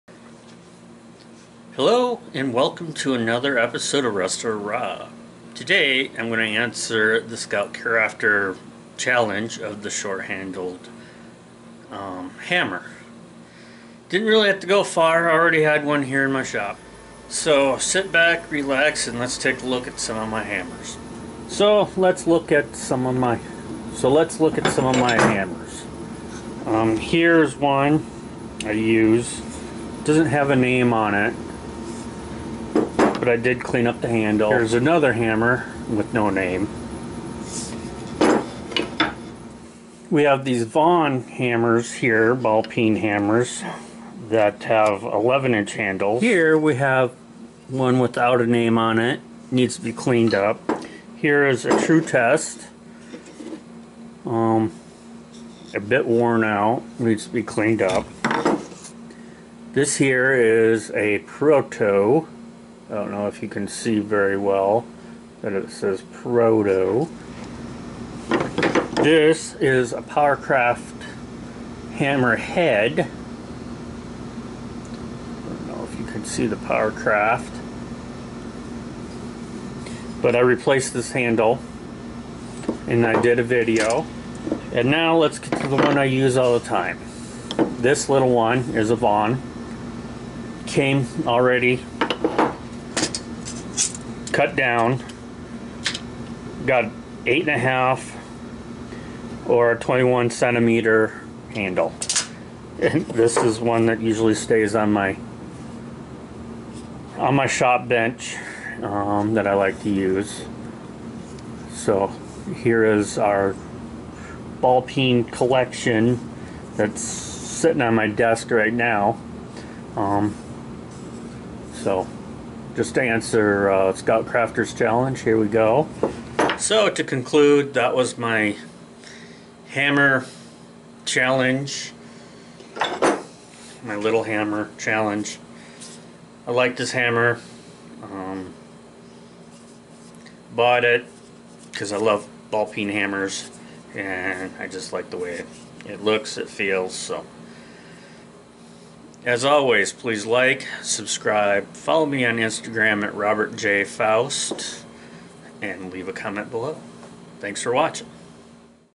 Scoutcrafter's Short Handle Hammer Challenge
A short video of my ballpeen hammer collection and my short handled hammer.

In the instance we include copy written material it is done so with consent of the owner.